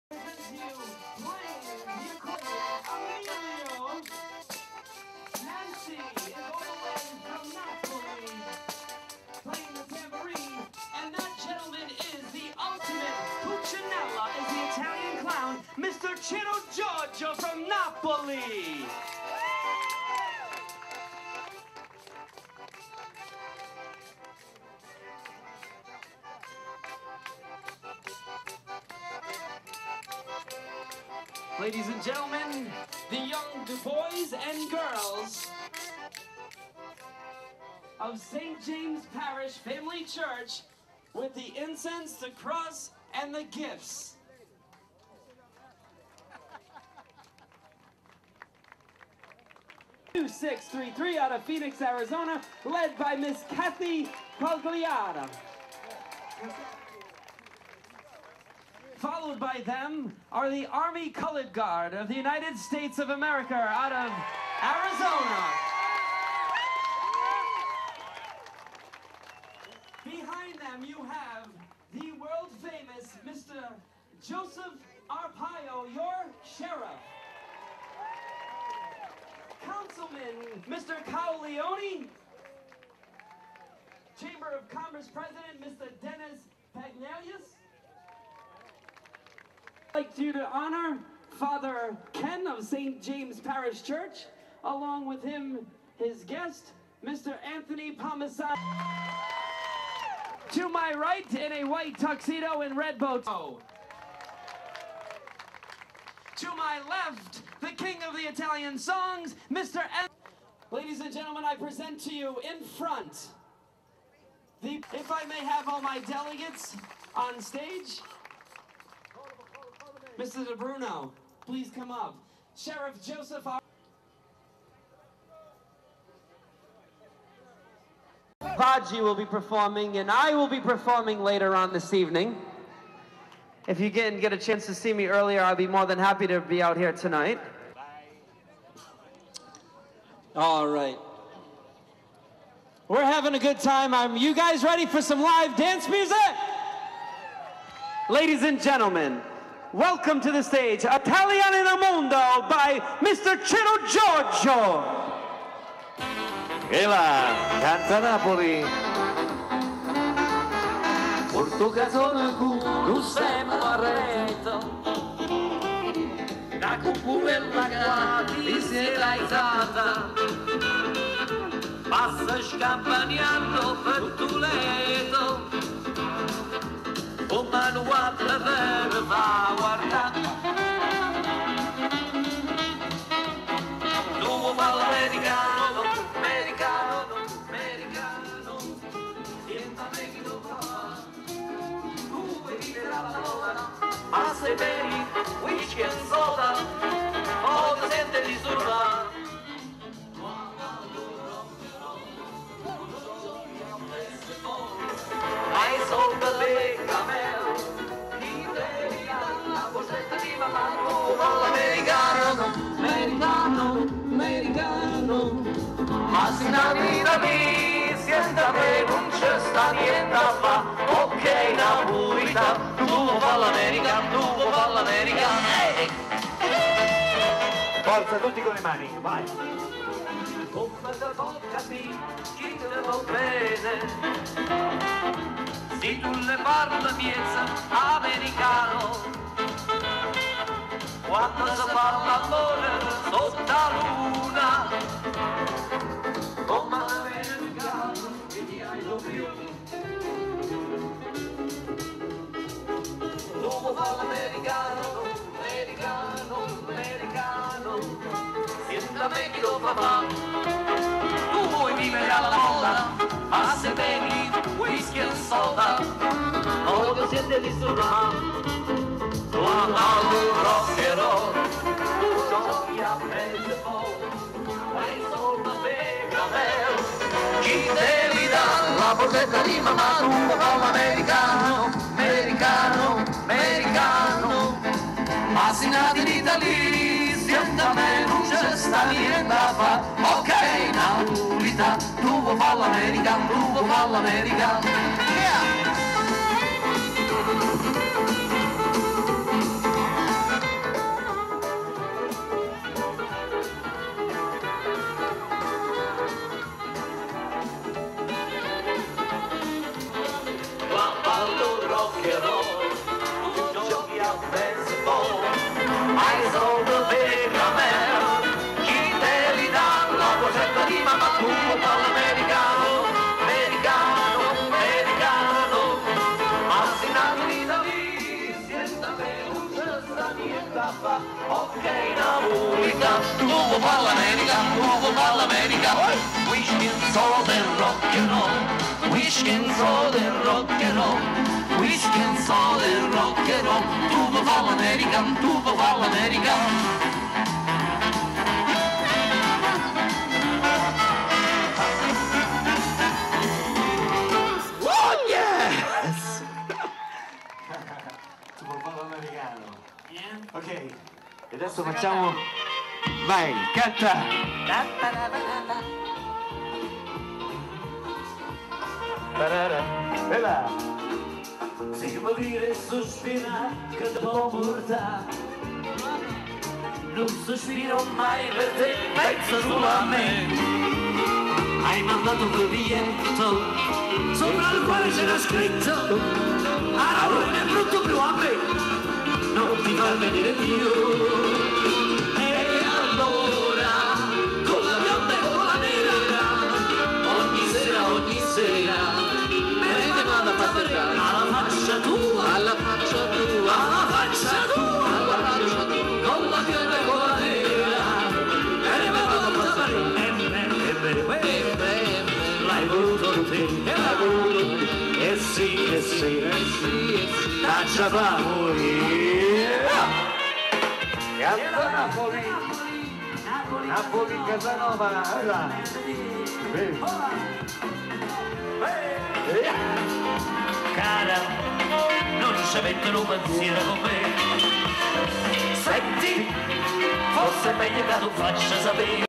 Peoria
Ciro Giorgio a Peoria Arizona  al San Gennaro Feast